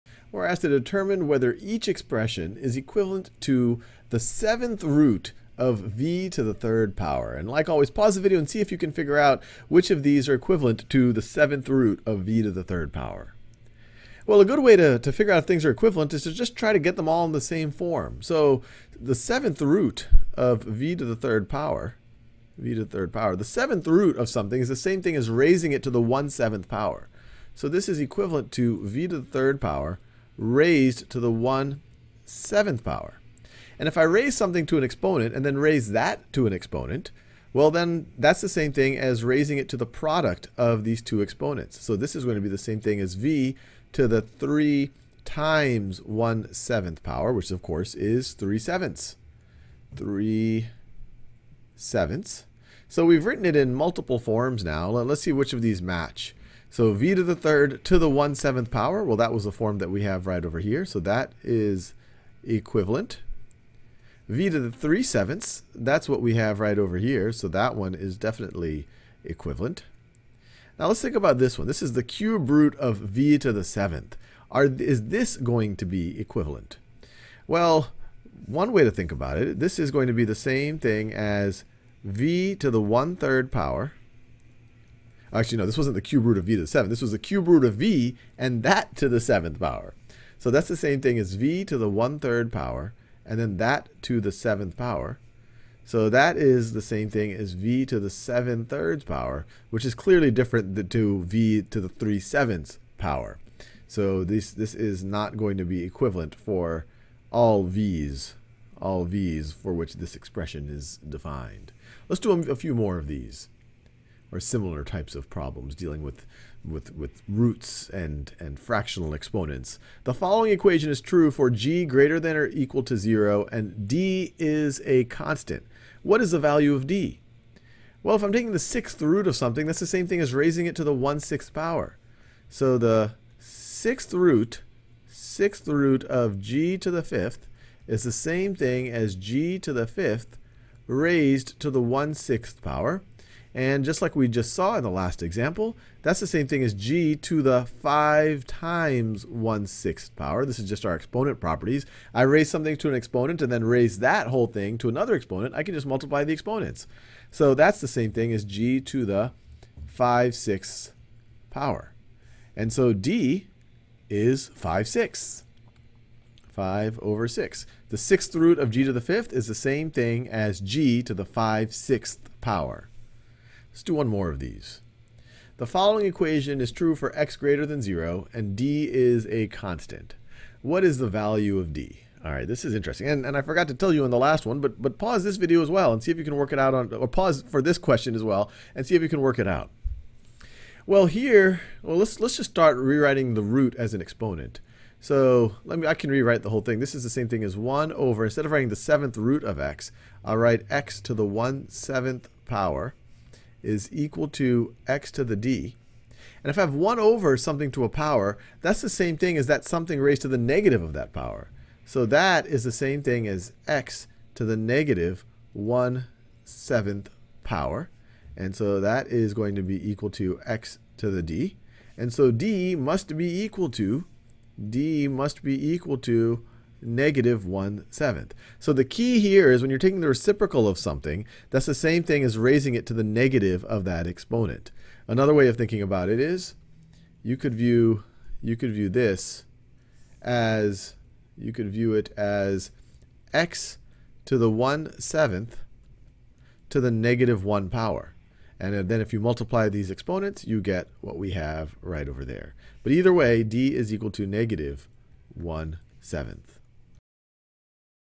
Rewriting roots as rational exponents | Mathematics I | High School Math | Khan Academy
Sal solves several problems about the equivalence of expressions with roots and rational exponents. For example, rewrite ⁶√(g⁵) as g^⅚.